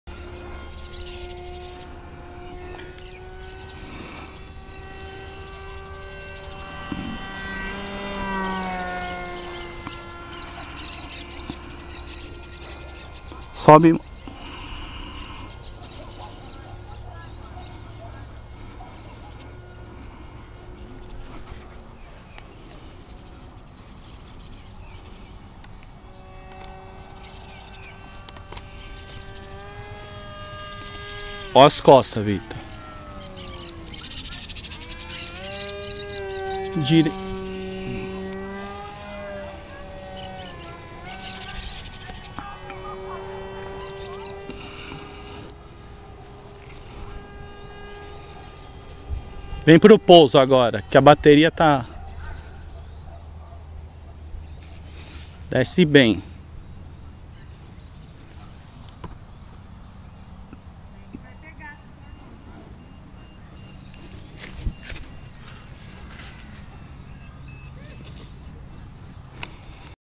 Pilotando,após Treino no Simulador
Depois de muito treino no simulador,voo em campo,
Meu filho ta ficando bom na Asa Zagui.